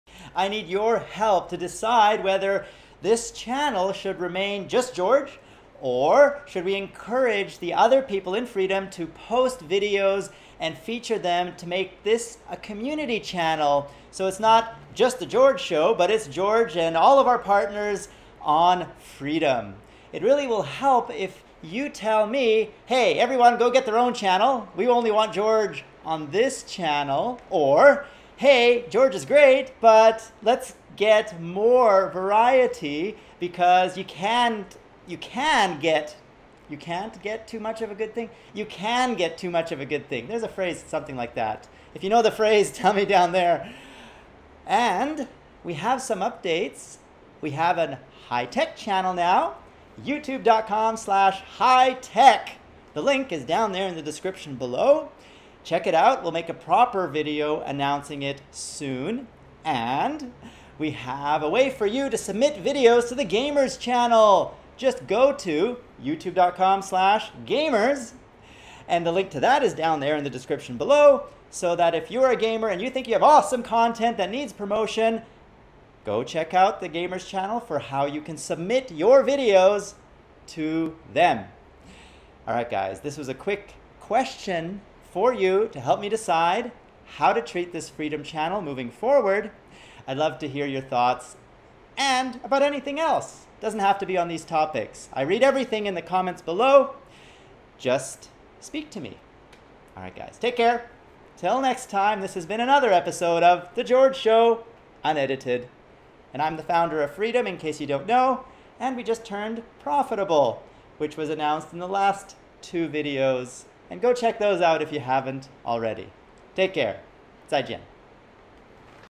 George-only on Freedom!? - Tell me!
Should this Freedom! channel become a community channel featuring the best of Freedom! partners, or should it stay George-exclusive?

XSplit